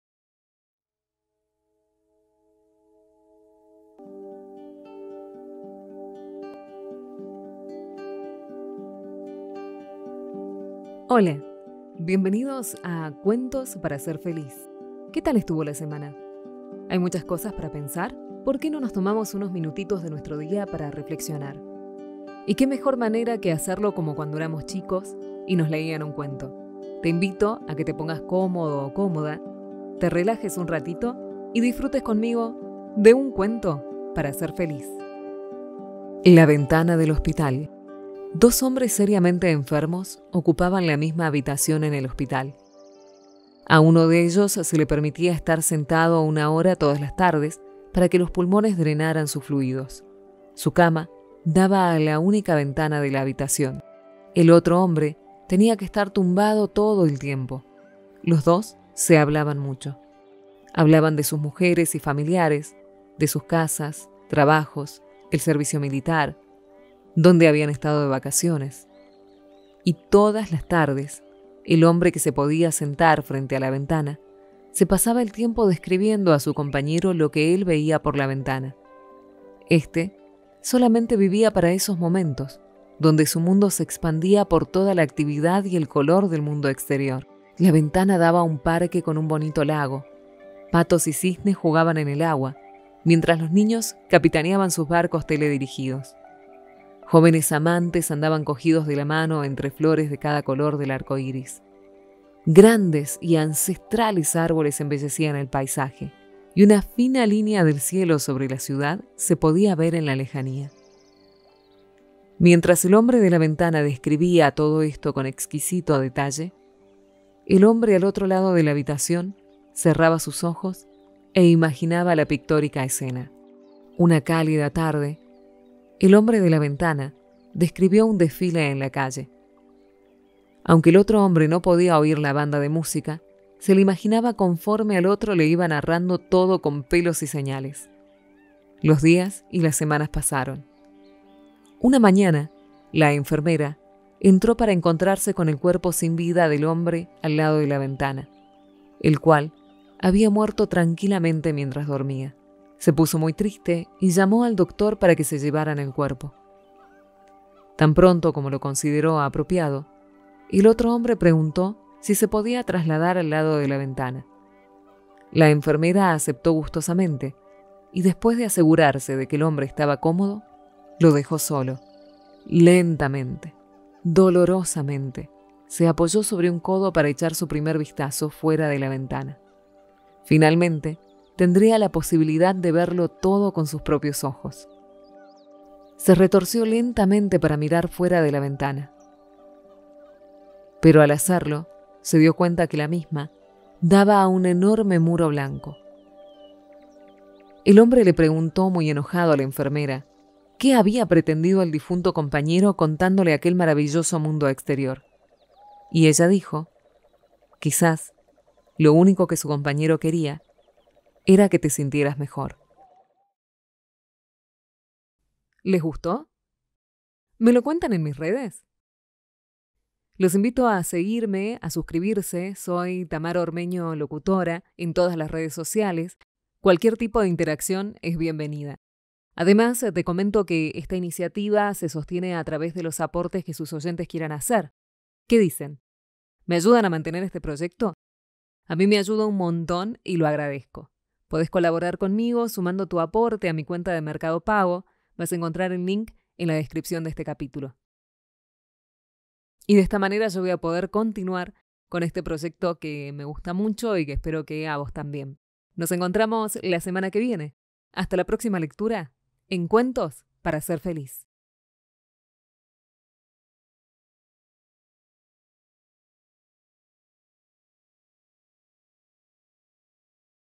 CUENTOS PARA SER FELIZ l CAPITULO 23: "LA VENTANA DEL HOSPITAL"
📚“Cuentos para Ser Feliz”

Capítulo 23: "LA VENTANA DEL HOSPITAL"

Este cuento anónimo, ‘La ventana del hospital’, nos habla de cómo con tan poco se puede conseguir tanto en otras personas. De cómo se puede, con imaginación, encender la chispa de la ilusión y la vida. Nos habla de bondad, compasión y pensamiento positivo. Nos habla de solidaridad y sobre todo, empatía. Un precioso cuento con valores para todas las edades, que te llenará por dentro.

👋 Soy Tamara Ormeño y empecé este proyecto porque me encanta narrar
¿Ya me seguís en las redes?

👋 ¡Charlemos!